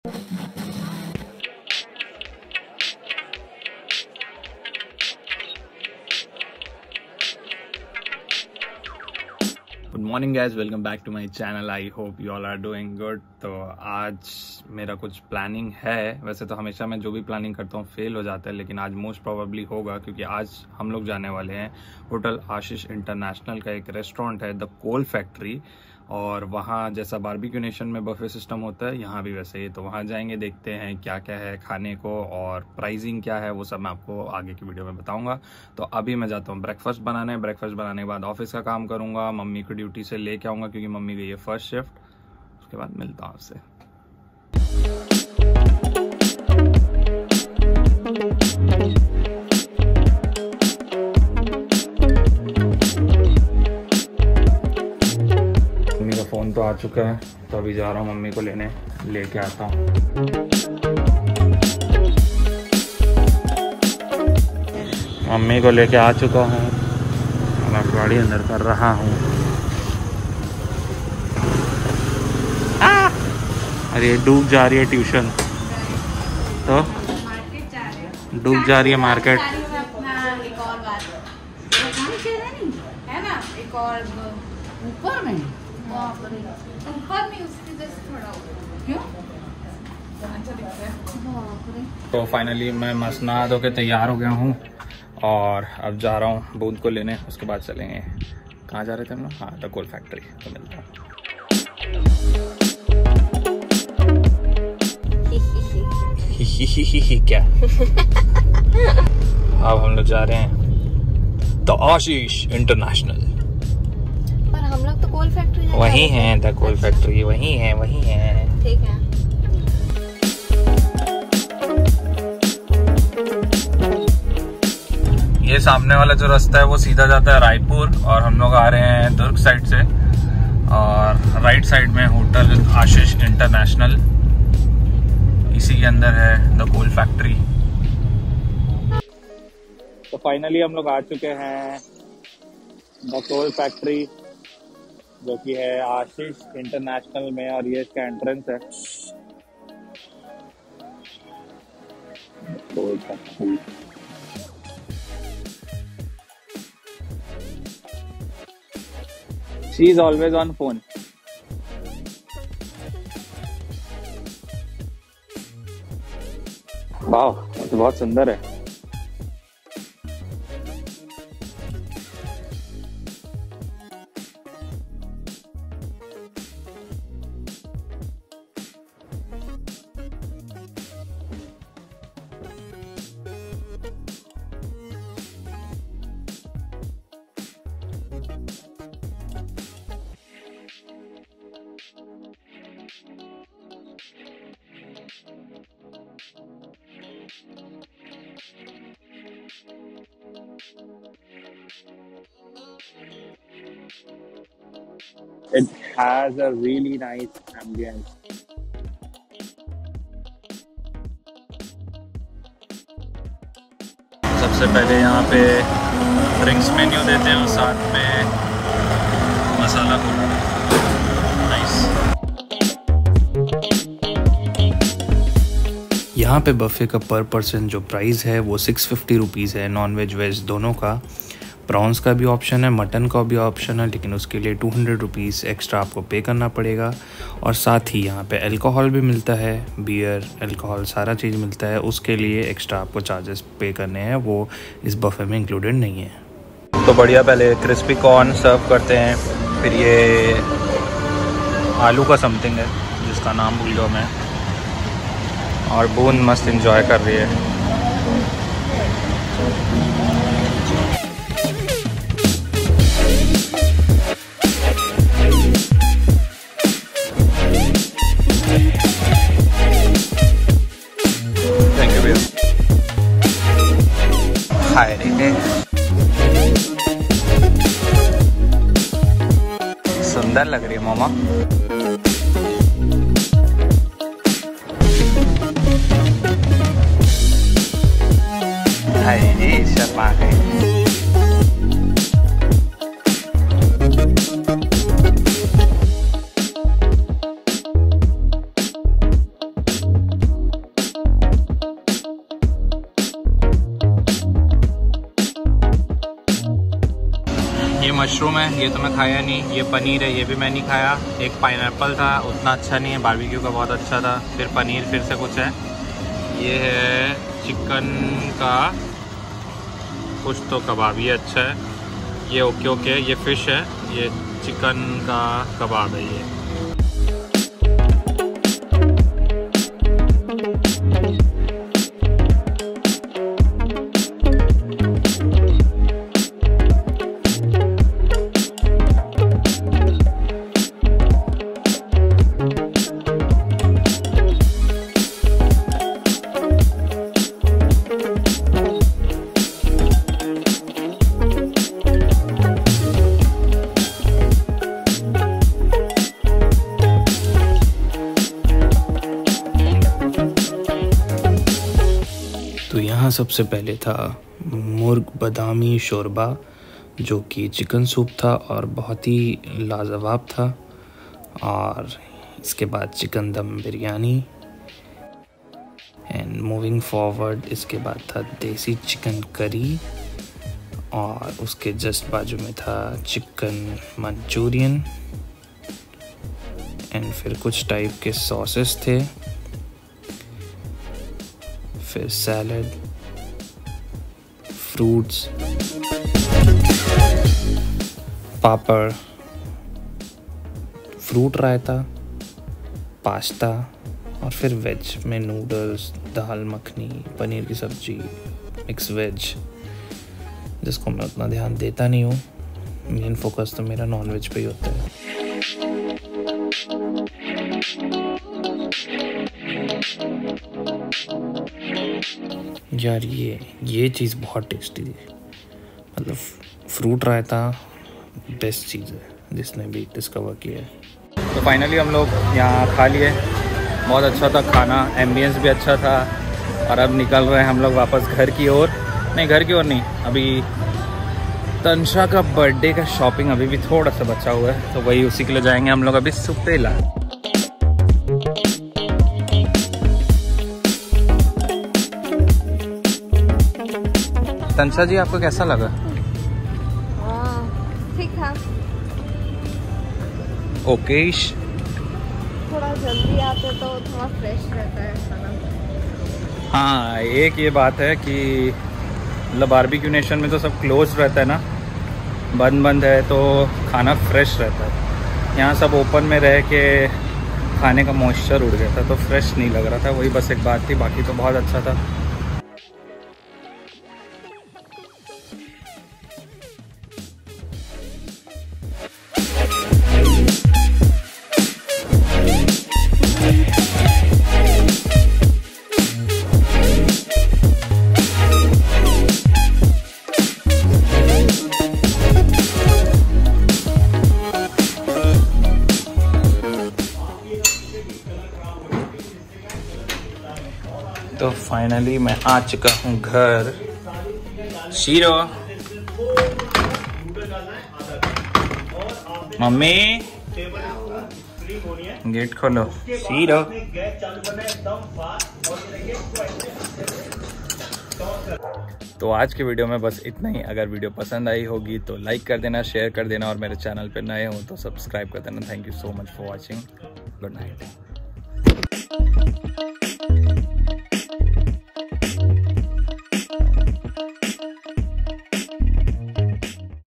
The Coal Factory Bhilai | अन्लिमटेड बफ़े @ ₹650 only | Vlog #26
Grills on table for the first time in Bhilai.
The Coal Factory is a buffet restaurant of Hotel Ashish International, GE Road, Powerhouse, Bhilai

Price per person for veg/non-veg - ₹650
Prawns and mutton available for extra ₹200/person
*serves alcohol as well*

Monday to Saturday - 5 pm onwards
Sunday - 1 pm onwards